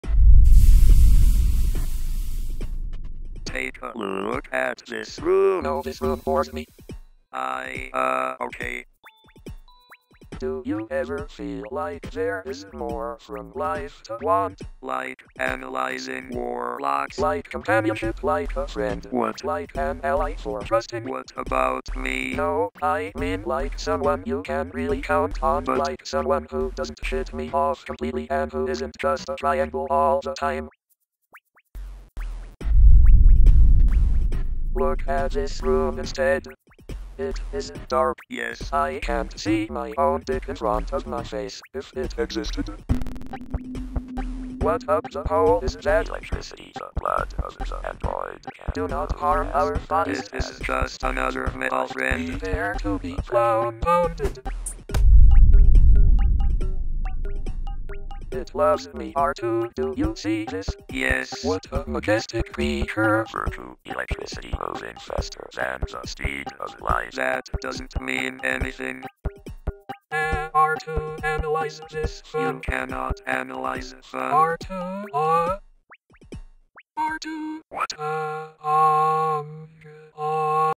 NotWizards Episode 5
EPISODE 5:
DIRECT CURRENT

it is very hot in melbourne today
to celebrate enjoy some spicy hot robot animation
and why enjoy these spicy hot robots all alone?
tell your friends about the robots you selfish bastard